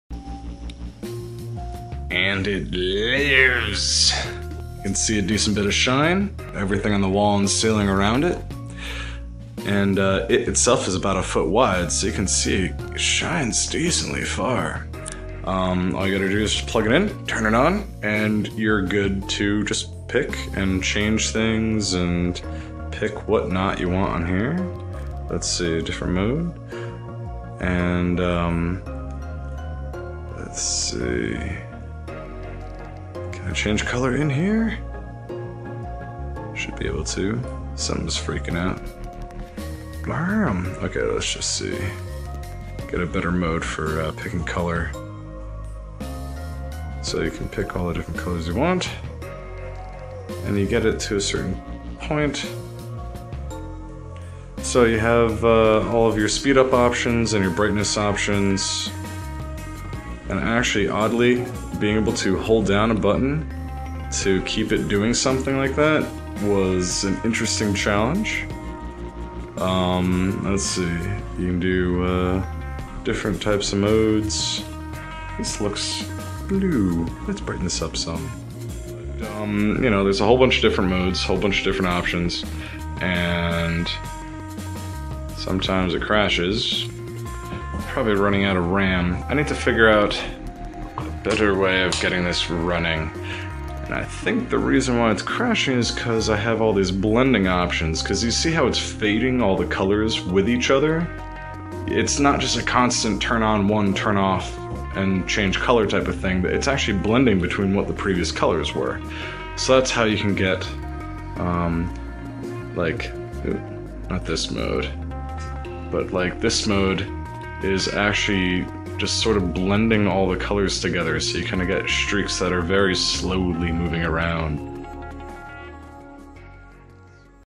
Spiral Lock: Color Mount
Finally stand alone!  Just plug it in and turn it on!
I still need to finish programming some of the modes.  There are some options that crash the system or just... don't look cool. Hah.

I know I keep posting these videos in segments. But I don't really want to wait to mix all these videos together to make one video.
Yeah yeah... I just want to show people.